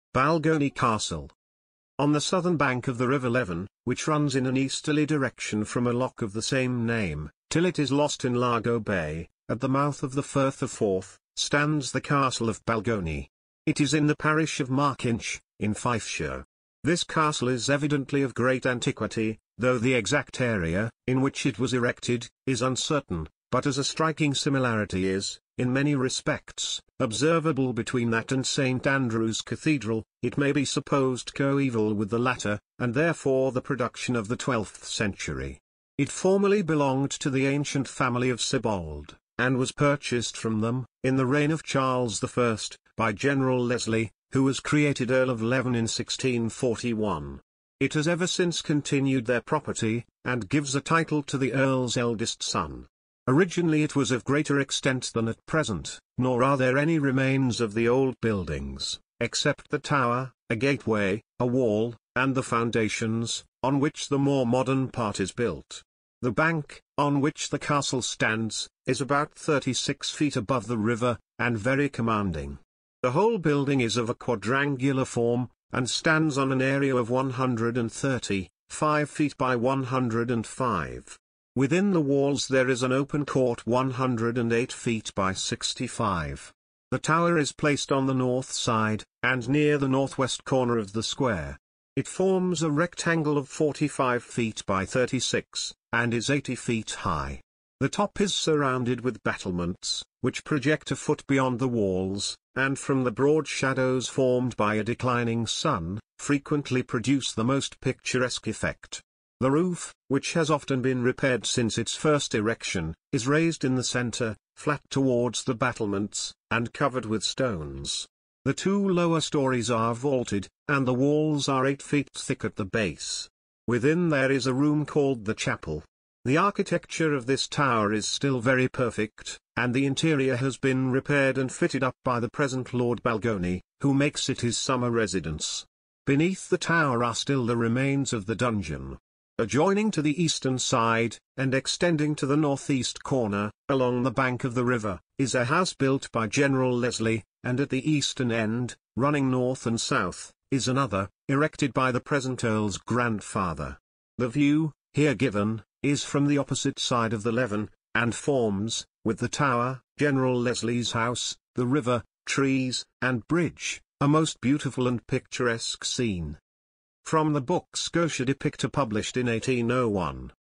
Balgonie Castle
On the southern bank of the River Leven, which runs in an easterly direction from a Loch of the same name, till it is lost in Largo Bay, at the mouth of the Firth of Forth , stands the Castle of Balgonie. It is in the parish of Markinch , in Fifeshire. This castle is evidently of great antiquity, though the exact area,
in which it was erected , is uncertain : but as a striking similarity is, in many respects, observable between
that and St. Andrew's Cathedral, it may be supposed coeval with the latter, and therefore the production
of the twelfth century. It formerly belonged to the ancient family of Sibbald , and was purchased from
them , in the reign of Charles I, by General Leslie, who was created Earl of Leven in 1641. It has ever
since continued their property, and gives a title to the earl's eldest son. Originally it was of greater extent
than at present; nor are there any remains of the old buildings, except the tower, a gateway, a wall, and
the foundations , on which the more modern part is built. The bank, on which the castle stands, is about
thirty -six feet above the river, and very commanding. The whole building is of a quadrangular form , and
stands on an area of one hundred and thirty - five feet by one hundred and five . Within the walls there is
an open court one hundred and eight feet by sixty-five. The tower is placed on the north side, and near
the north-west corner of the square. It forms a rectangle of forty -five feet by thirty-six , and is eighty feet
high . The top is surrounded with battlements, which project a foot beyond the walls, and from the broad
shadows formed by a declining sun, frequently produce the most picturesque effect. The roof, which has
often been repaired since its first erection , is raised in the centre, flat towards the battlements, and covered
with stones . The two lower stories are vaulted , and the walls are eight feet thick at the base. Within
there is a room called the chapel. The architecture of this tower is still very perfect, and the interior has
been repaired and fitted up by the present Lord Balgonie , who makes it his summer residence. Beneath
the tower are still the remains of the dungeon . Adjoining to the eastern side, and extending to the north
east corner , along the bank of the river, is a house built by General Leslie ; and at the eastern end, running
north and south , is another, erected by the present Earl's grandfather. The view , here given , is from the
opposite side of the Leven, and forms, with the tower, General Leslie's house, the river, trees, and bridge,
a most beautiful and picturesque scene.

From the book Scotia Depicta published in 1801